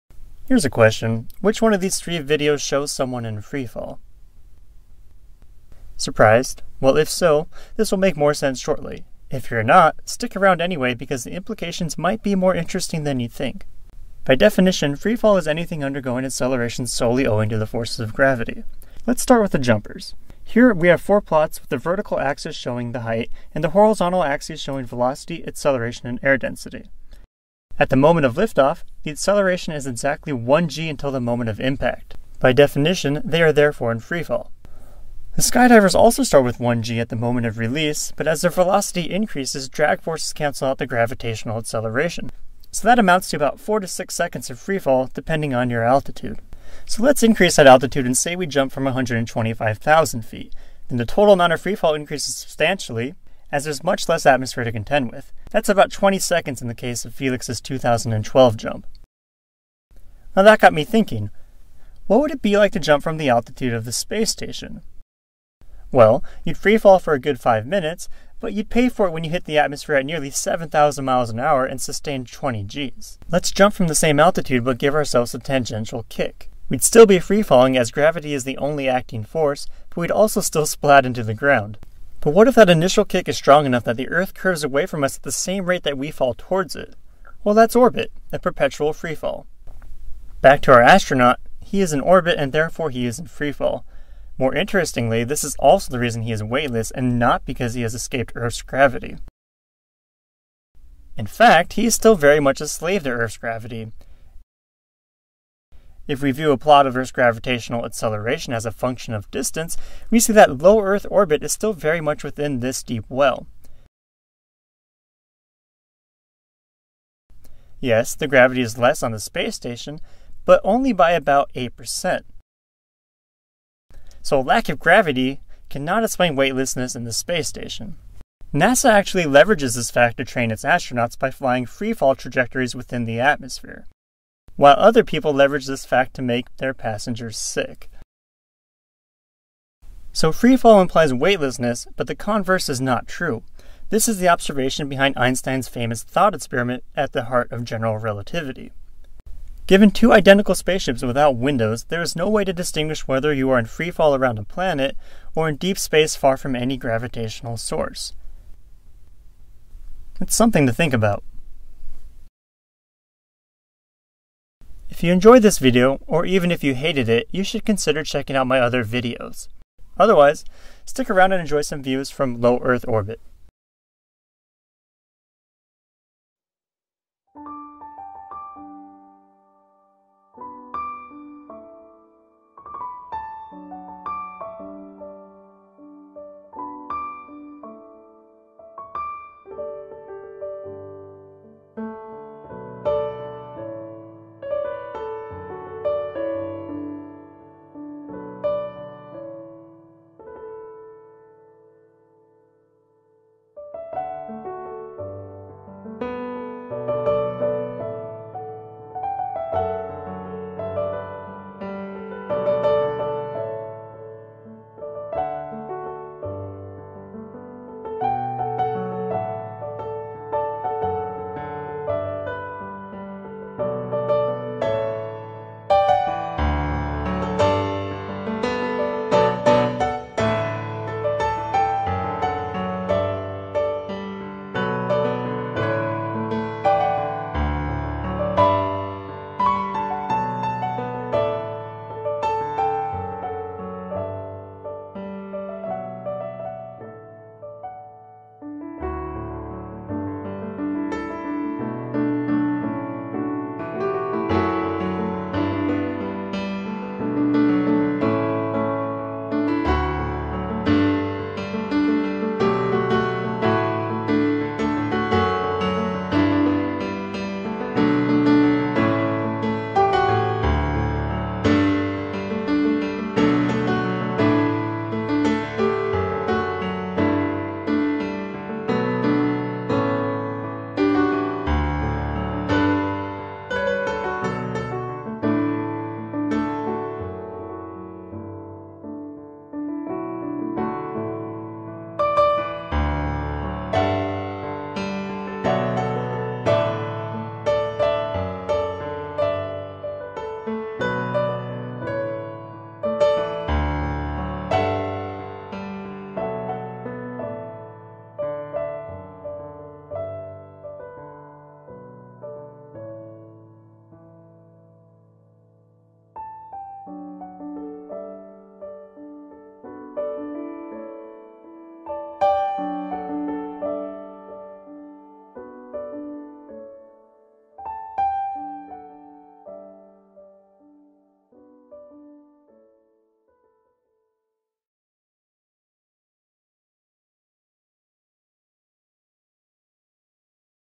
Freefall, Astronauts and General Relativity
This video describes exactly what we mean by "freefall" and its implications in regards to astronauts and Einstein's general theory of relativity.

If you want to skip all the science stuff, skip to the 3-minute mark for some timelapse footage aboard the international space station.

Credits:
Figures/Plots were created using MATLAB
Timelapse videos came straight from NASA